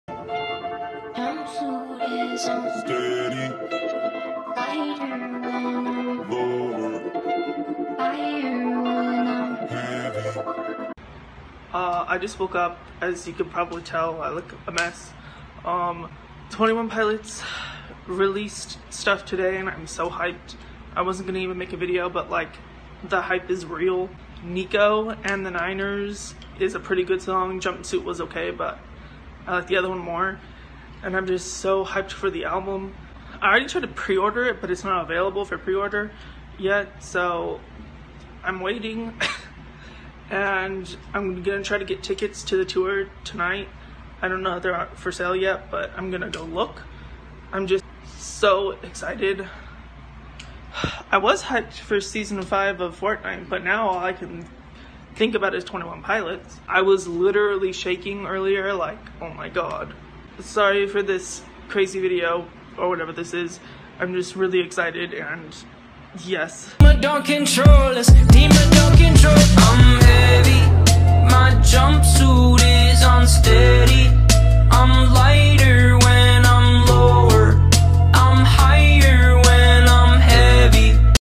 Soooooo Twenty One Pilots Is Back
I am so pumped for the new era of the beans and can't wait for more music and the tour, I was literally shaking when the videos showed up in my feed